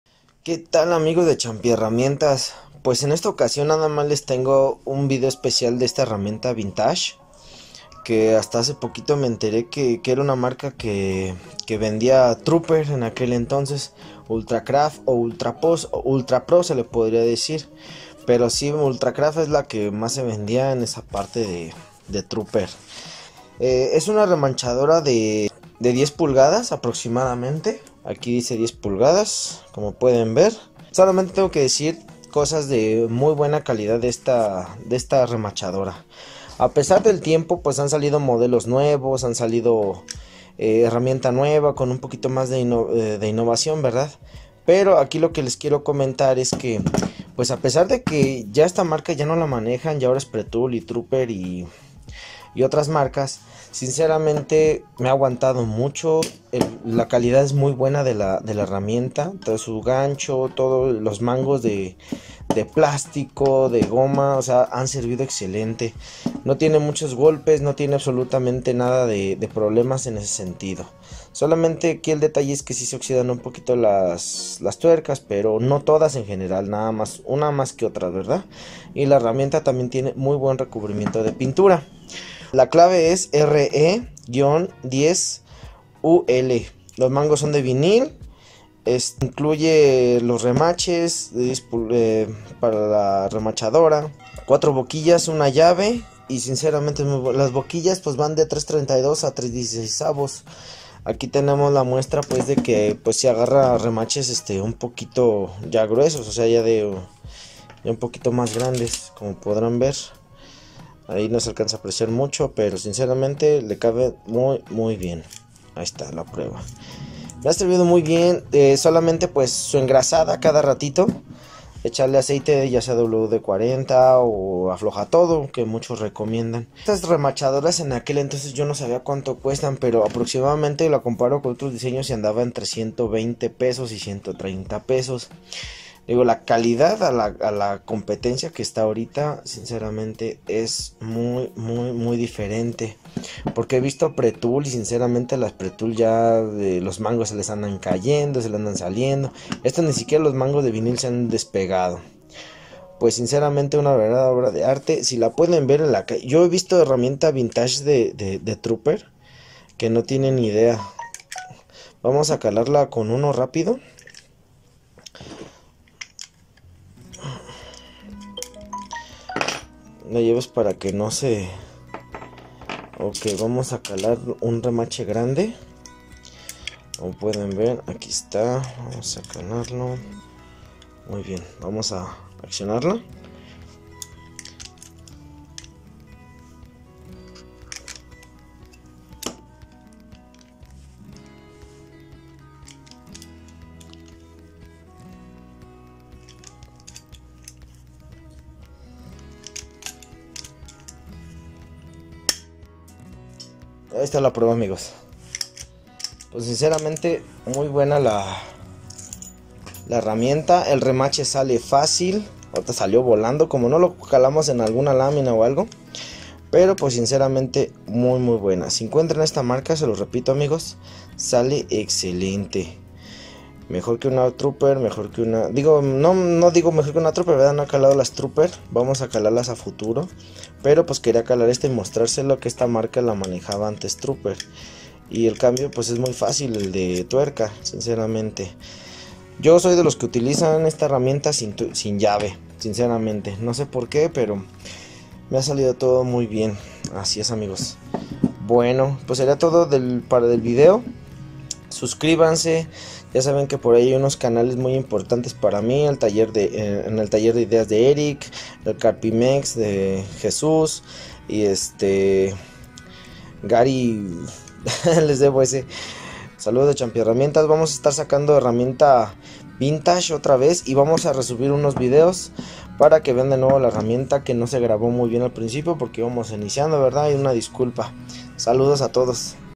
REMACHADORA ULTRACRAFT (OTRA MARCA DE TRUPER)
UNA REMACHADORA VIEJITA, PERO QUE NO TIENEN IDEA COMO SE DEFENDIO AHORA ESTA CON ALGUIEN MAS POR QUE LE GUSTO TANTO SU DESEMPEÑO QUE NO ME ACEPTARON LA TRUPER EN VERDAD UNA MUY BUENA OPCION LES RECOMIENDO ESTA REMACHADORA SI LA VEN SE VAN A QUEDAR IMPRESIONADOS SALUDOS AMIGOS A TODOS.